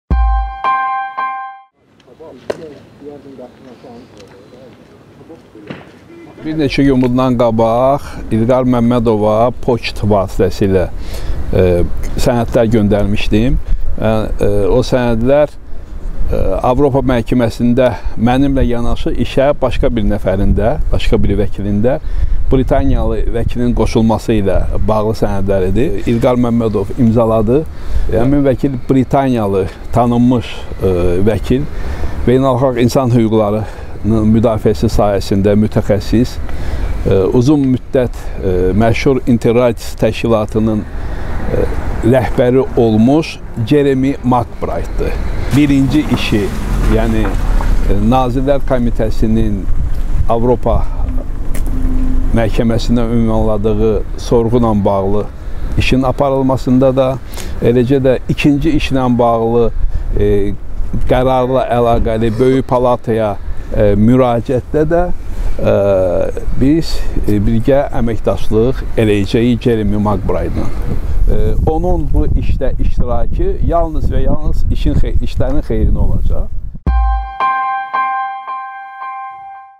İlqar Məmmədovun işinə britaniyalı vəkil qoşulub
ReAL-ın həbsdə olan sədri İlqar Məmmədovun işinə Böyük Britaniyalı vəkil Ceremi Makfrayt da qoşulub.
Bu barədə vəkil Fuad Ağayev yanvarın 19-da 2 saylı Cəzaçəkmə Müəssisəsinin qarşısında keçirdiyi brifinqdə bildirib.

O qeyd edib ki, Ceremi Makbrayt insan hüquqları sahəsində beynəlxalq təcrübəyə malikdir, uzun müddət məşhur İnteradiz təşkilatının rəhbəri olub. Hazırda o, Ukraynanın Rusiyaya qarşı işi aparır. Onun İlqar Məmmədovun müdafiəsinə qoşulması işlərin xeyrinə olacaq:
“Mən ingilis dilində bilsəm də, peşəkar sayıla bilmərəm. Əsas məsələ Ceremi Makbraytın bu sahədə peşəkarlığıdır”, - F.Ağayev deyib.
Vəkil daha sonra deyib ki, o, ötən ilin sonlarından İlqar Məmmədovun yanına gələrkən yoxlamaya məruz qalır:
“Bu dəfə də ona göndərdiyim sənədlər bir sutkadan sonra verilib. Vəkillə müdafiə etdiyi şəxs arasında yazışmalar konfidensial xarakter daşımalı və senzuraya məruz qalmamalıdır. Bu xoşagəlməz yoxlama adi xarakter almağa başlanıb”.
O, bundan sonra İlqar Məmmədovun işi bağlı nə gözlədiyi sualına “proqnoz vermirəm” deyib və prosesdə heç bir dəyişikliyin olmayacağını əlavə edib.
İ.Məmmədov 2013-cü ilin yanvarında İsmayıllıda kütləvi iğtişaşların təşkilində ittiham olunaraq, 7 il azadlıqdan məhrum edilib.
Avropa İnsan Hüquqları Məhkəməsi (AİHM) 2014-cü ilin mayında İ.Məmmədovun azadlıq hüququ və təqsirsizlik prezumpsiyasının pozulmasını tanıyıb.
Avropa Şurası Nazirlər Komitəsi Azərbaycana qarşı Avropa Konvensiyasının 46 maddəsi ilə öz öhdəliyini yerinə yetirməmə məsələsini qaldırıb. Avropa Şurasının 68 illik tarixində hələ heç bir ölkəyə 46-cı maddə ilə öz öhdəliyini yerinə yetirməmə məsələsi tətbiq olunmayıb. Ona görə həmin prosedurun başlamasının Azərbaycandan ötrü hansı nəticələri doğuracağını hələlik heç kəs konkret söyləyə bilmir.